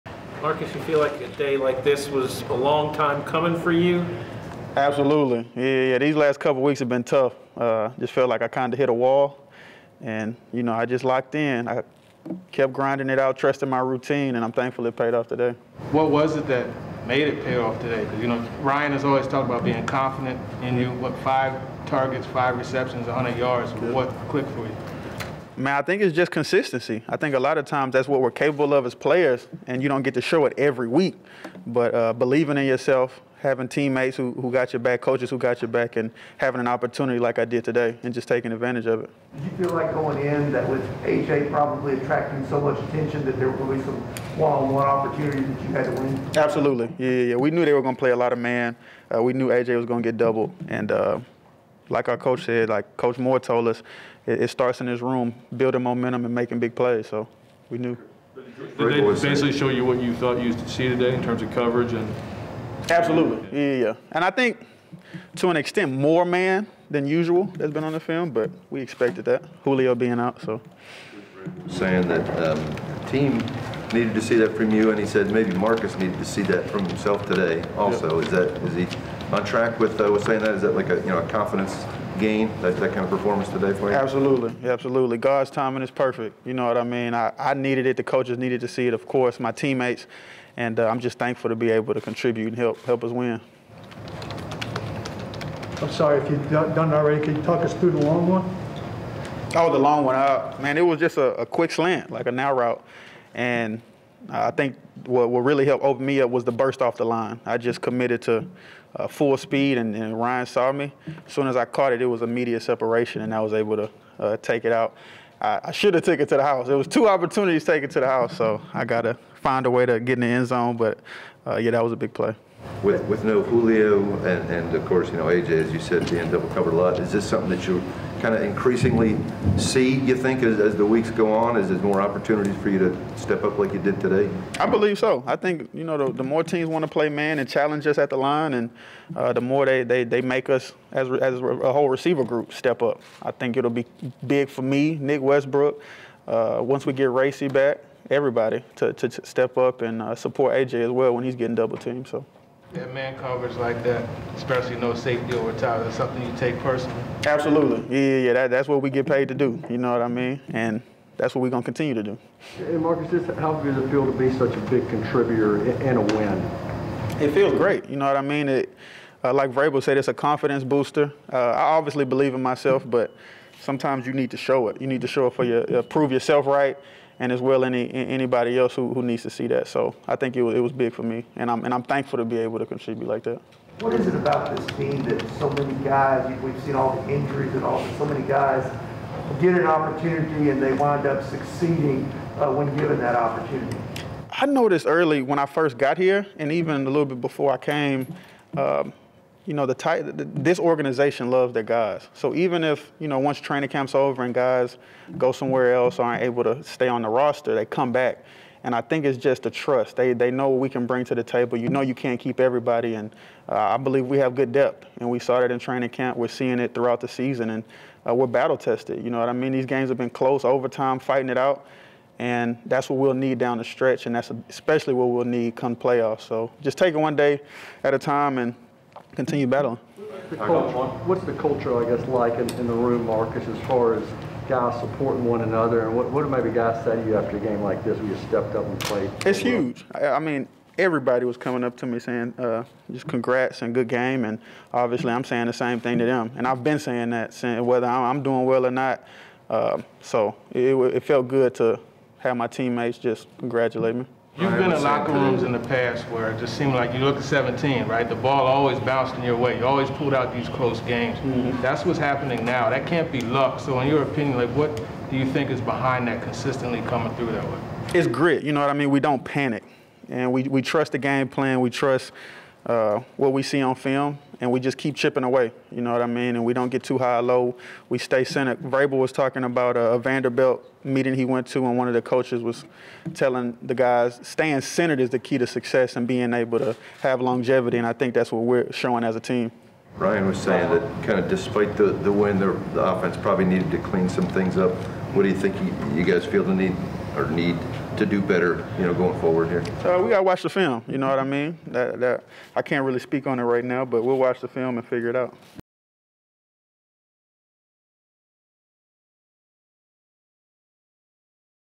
Staying Centered is the Key to Success | Marcus Johnson Player Interview
Titans WR Marcus Johnson addresses reporters following Tennessee's 23-21 victory over the New Orleans Saints on Sunday at Nissan Stadium.